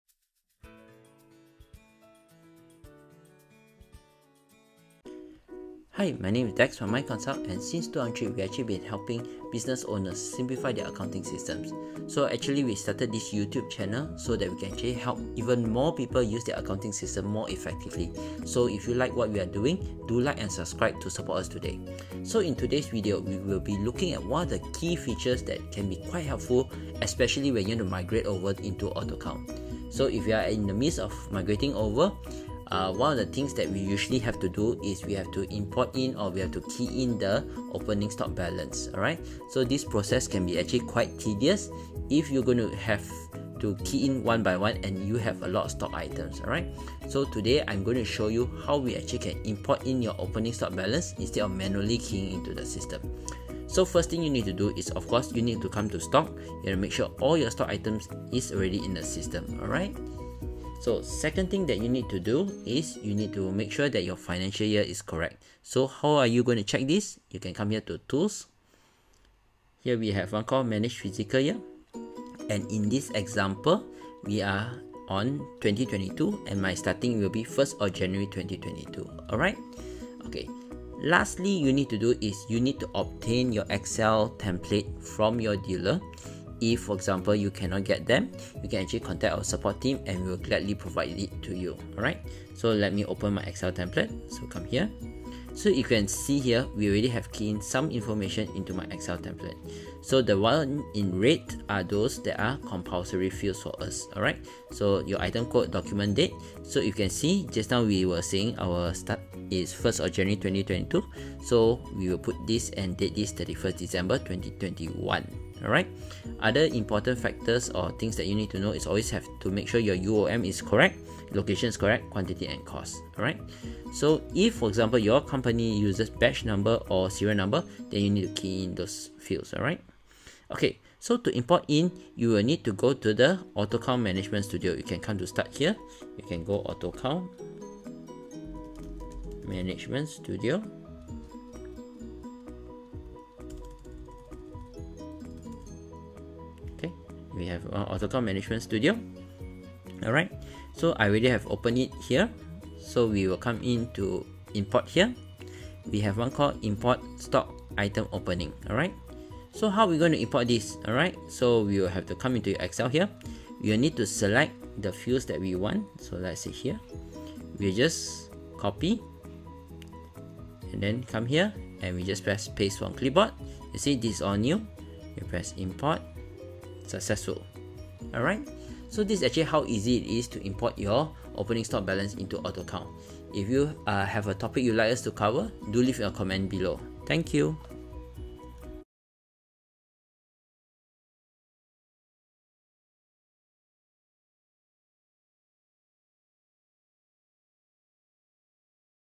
AutoCount - How to Import your Opening Stock Balance into AutoCount
🚀 Unlock Time-Saving Efficiency in AutoCount! Learn How to Import Opening Stock Balance with Ease! 📊💻

Are you making the switch to AutoCount from another accounting system? Say goodbye to tedious manual data entry! In this tutorial, we'll guide you step-by-step on importing your opening stock balance seamlessly using a handy Excel template.

⏰ Save Time, Reduce Manual Work!
Migrating your data has never been smoother. Our tutorial ensures a hassle-free transition, allowing you to focus on what matters most – managing your business.

📈 Boost Productivity with AutoCount!
Discover the power of automation and unlock the potential of AutoCount for your business. Importing opening stock balances has never been this efficient!

📚 What You'll Learn:

Downloading the AutoCount Excel template
Formatting your data for seamless import
Uploading opening stock balance effortlessly